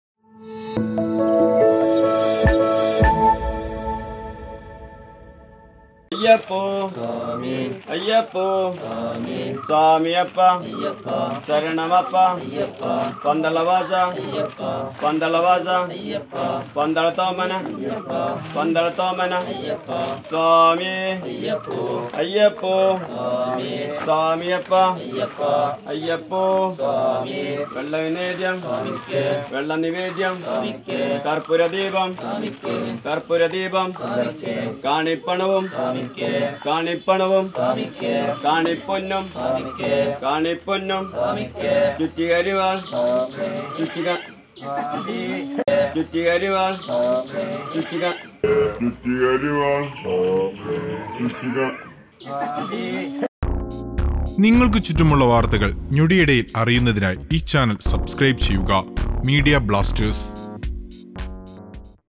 ശരണം വിളി പാളി പോയി.. ആ സമയം ഗുരുസ്വാമീടെ ഒരു ഞെട്ടൽ ഉണ്ട്..അതാണ് കിടിലൻ
Watch ശരണം വിളി പാളി പോയി.. ആ സമയം ഗുരുസ്വാമീടെ ഒരു ഞെട്ടൽ ഉണ്ട്..അതാണ് കിടിലൻ